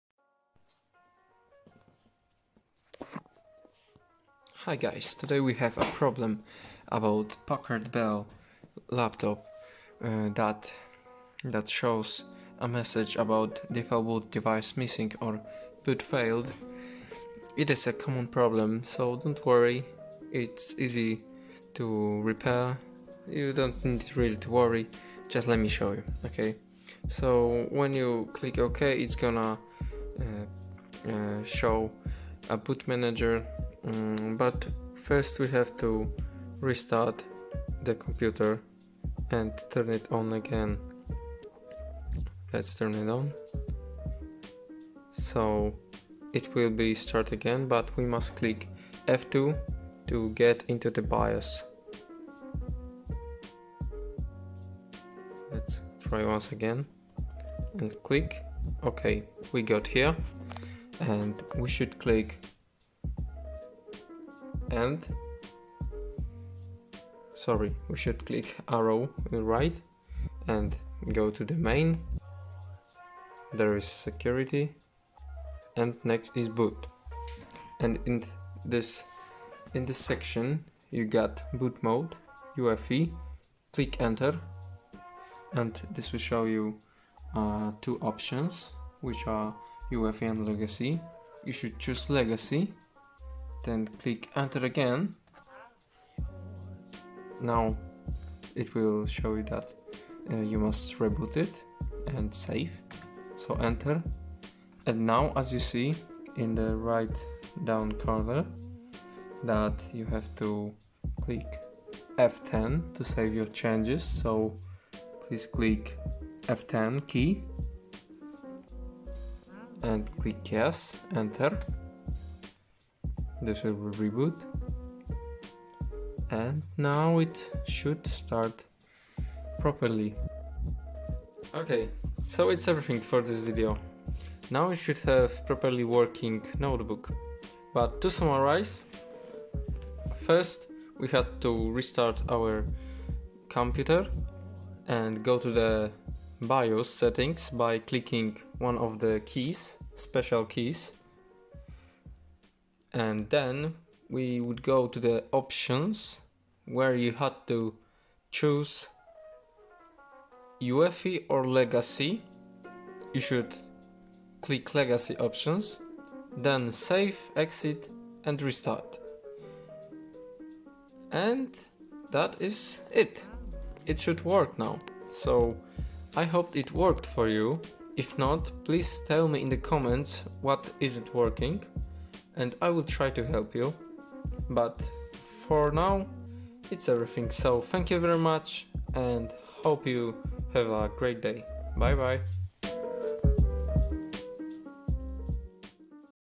[SOLVED] Default Boot Device Missing or Boot Failed. [Packard bell Acer Lenovo]
I hope it worked for you. If not, check if you have your Storage Drive (HDD or SSD) with Windows connected. Otherwise try installing Windows.

Default Boot Device Missing or Boot Failed. Insert Recovery Media and Hit any key Then Select 'Boot Manager' to choose a new Boot Device or to Boot Recovery Media. Lenovo, Packard bell, Acer, Dell, HP. Fix, repair. BIOS, UEFI, Legacy.